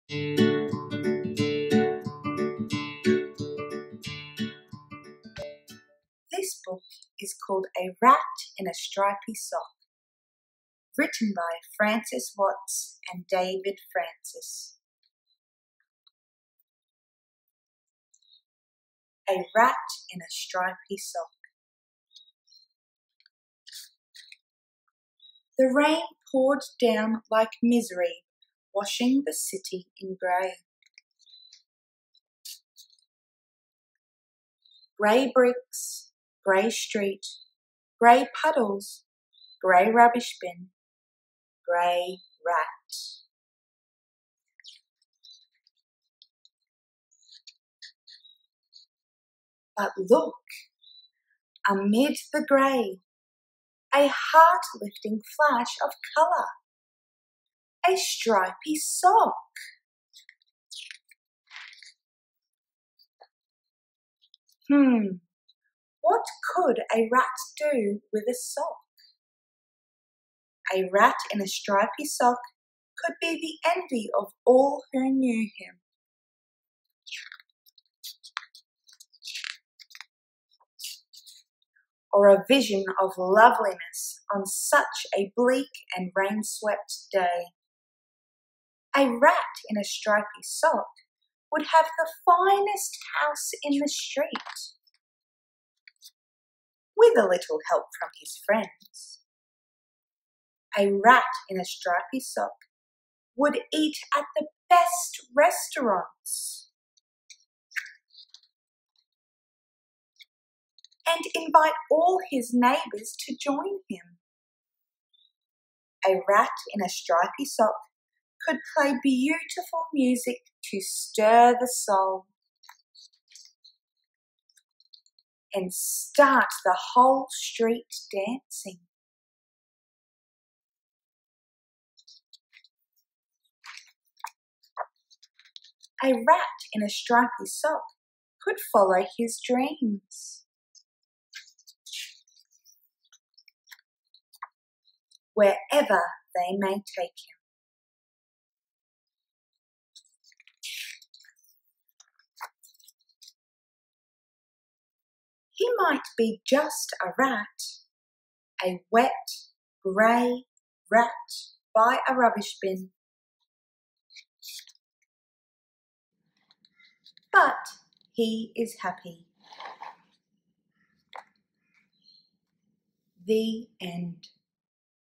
A RAT IN A STRIPY SOCK | read aloud | Teaching with Miss Fitz
Children's story "A rat in a stripy sock" by Frances Watts and David Francis. Share this Read Aloud, with your children or Kindergarten class. Read by Vanessa Fitzpatrick Appropriate for all ages.